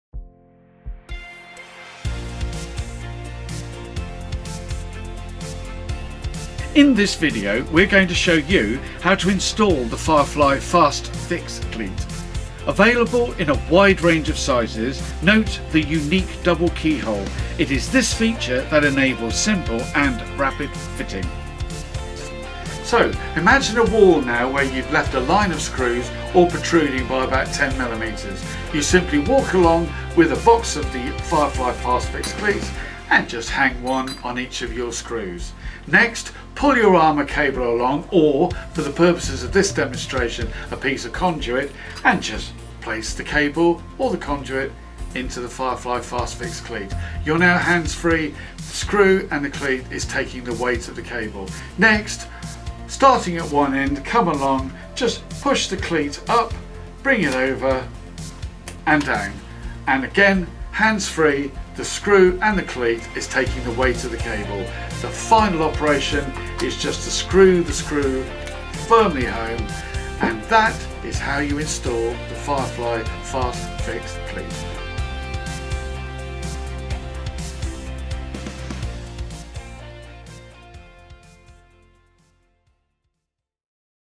Firefly Fire Resistant Metal Cable Cleat - Key Hole Fixing Version - How to
A short informative guide on how to fit our innovative faster fixing all metal cable cleats.
Providing compliance with the 18th edition along with peace of mind.